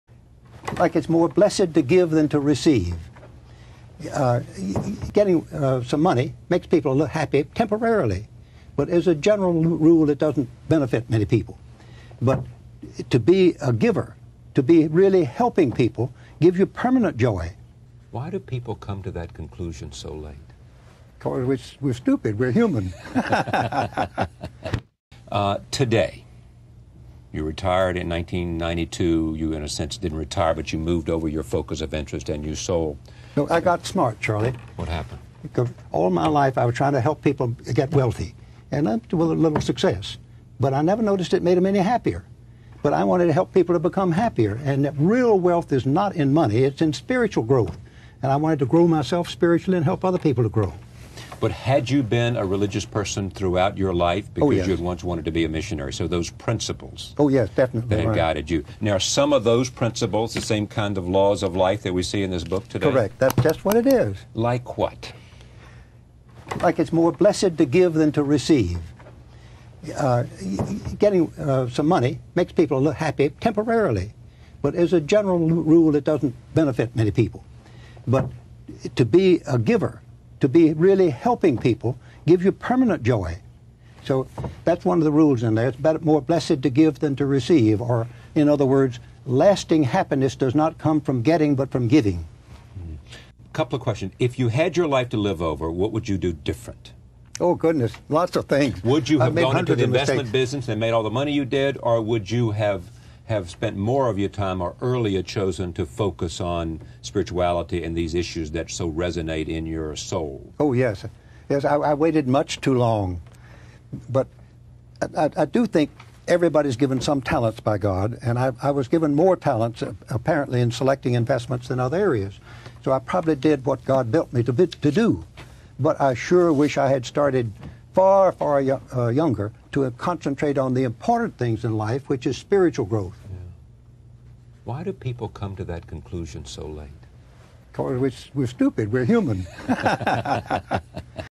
Real WEALTH is NOT in Money | John Templeton
John Templeton speaks on wealth, giving than taking, and the stupid human nature.

1. WATCH THE NEXT VIDEO 🎦

2. LEAVE YOUR IDEAS IN THE COMMENTS ✅
○ Did you like this video?
○ Is there anything you'd like to add?